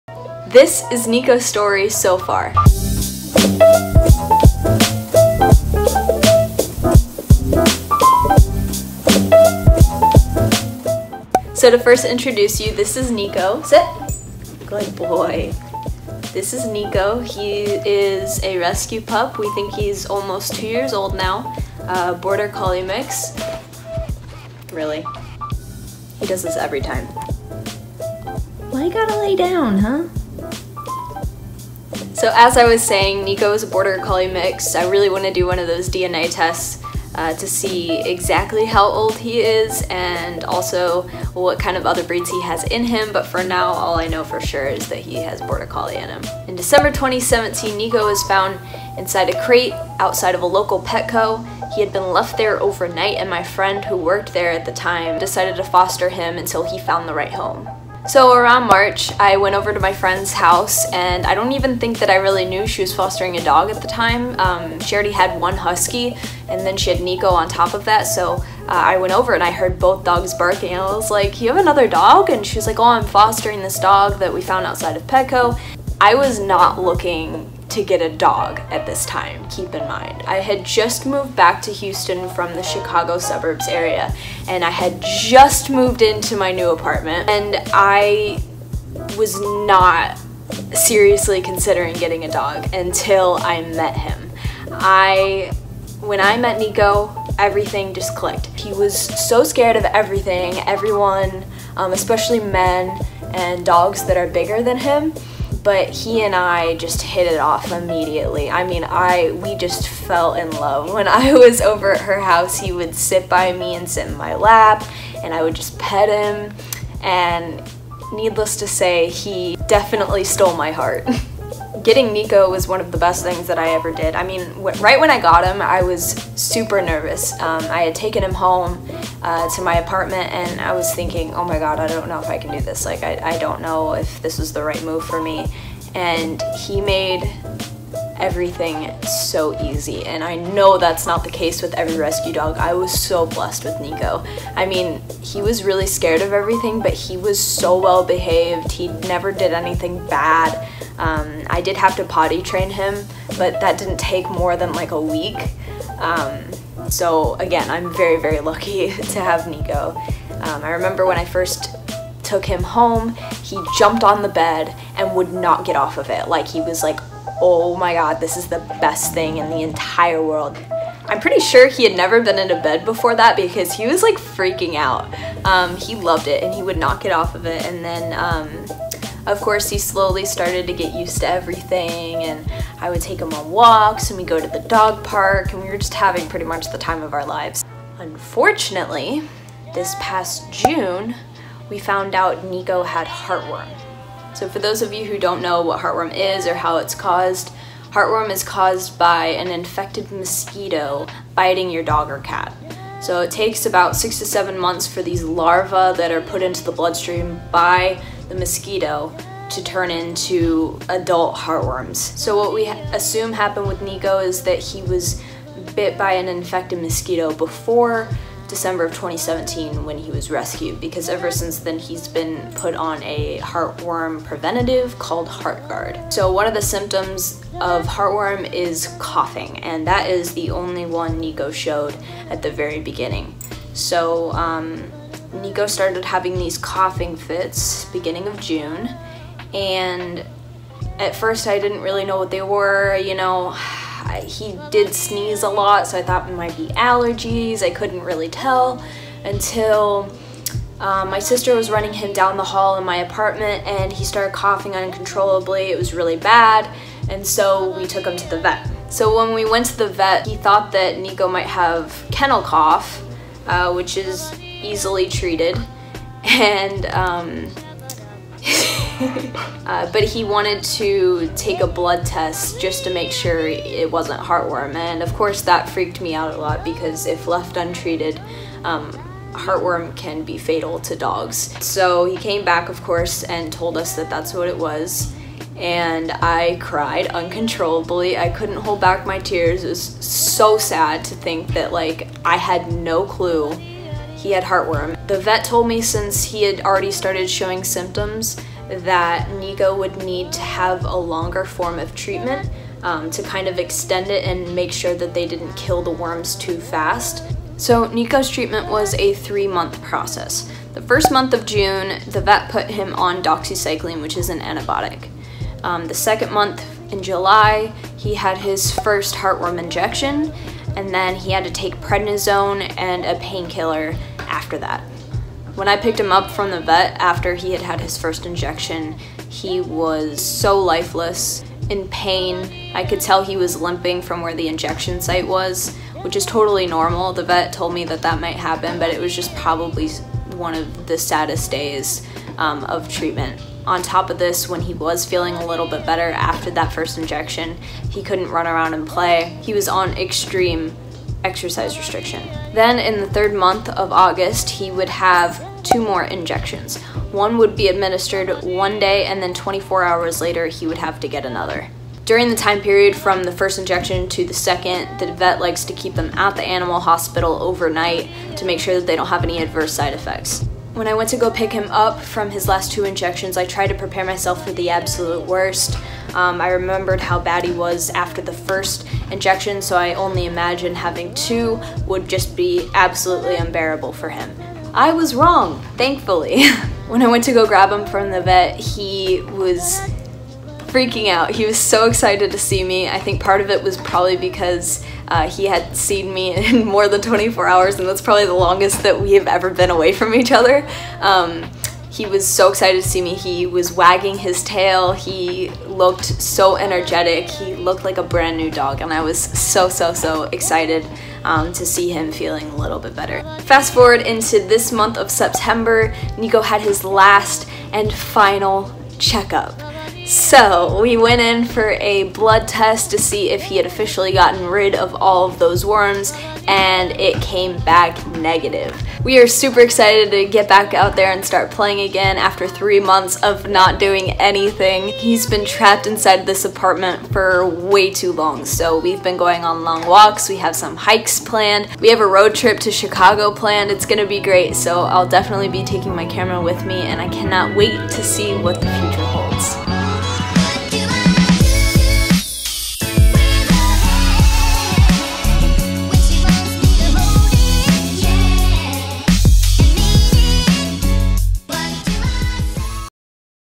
MY RESCUE DOG’S STORY + Heart Worm Treatment
Meet my rescue pup Niko!